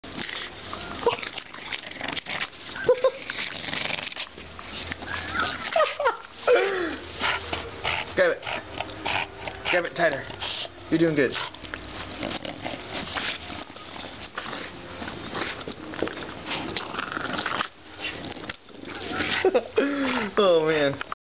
Bull Dog G-Force
MCRD San Diego's mascot rides the whip, proving that bulldogs CAN do something cool besides breathe like a chainsaw.